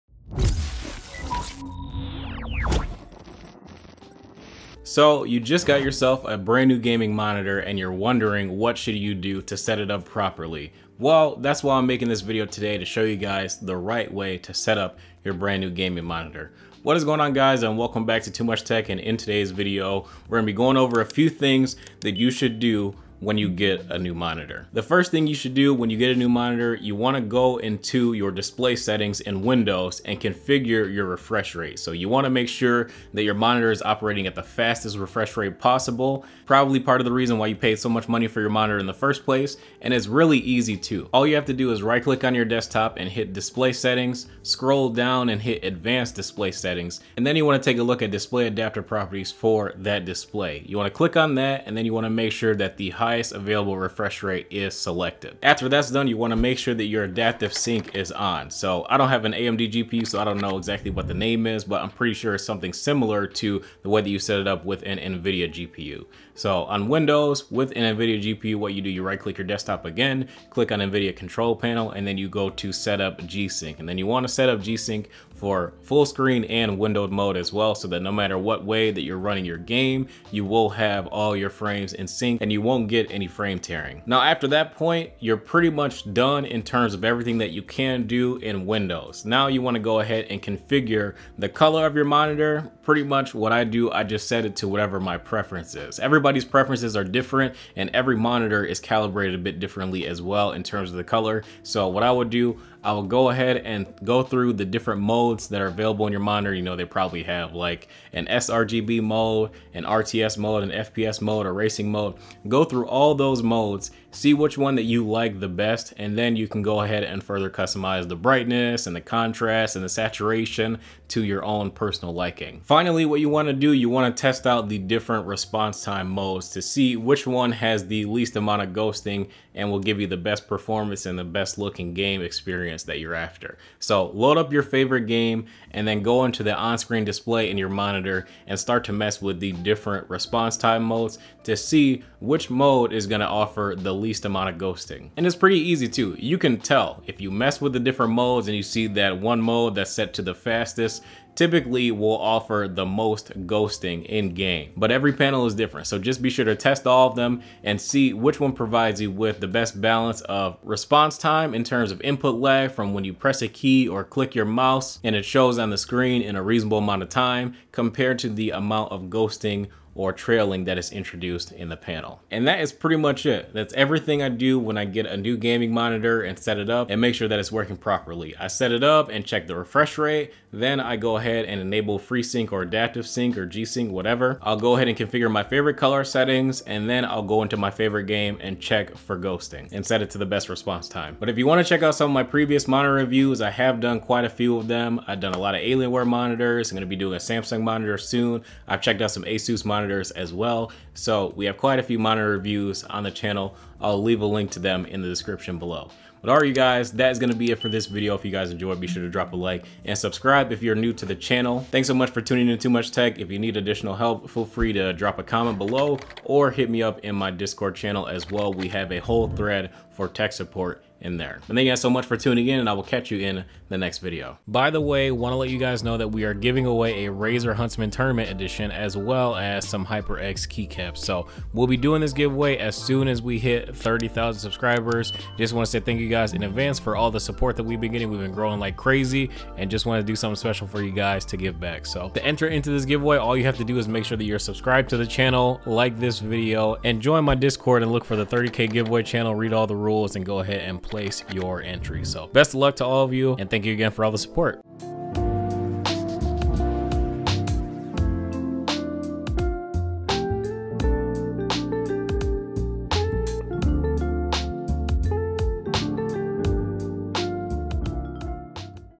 HOW TO PROPERLY Set Up Gaming Monitor! (BEST PERFORMANCE & Lowest Input Lag!)
Let me know if you have anything else special that you do to set up your new gaming monitors! This is just a quick checklist I do every time I plug one in!

Asus VG27AQ (1440p): (Amazon - paid link)